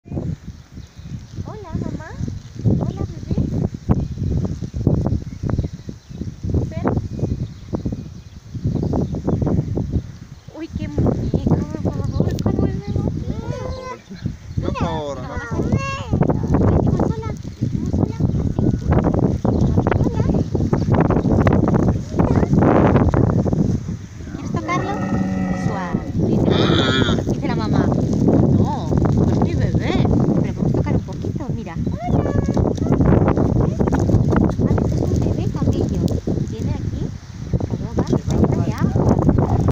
Le petit Chameau - Petit de la chamelle - Bébé dromadaire
Si vous êtes à Marrakech pour des vacances de détente et de loisirs, il est indispensable de découvrir la célèbre palmeraie de Marrakech.
faire une balade à la palmeraie de Marrakech afin de découvrir cette oasis, en famille, en couple, entre amis ous famille ..
Bébé dromadaire
Petit de la chamelle
Le petit Chameau
Chamelon.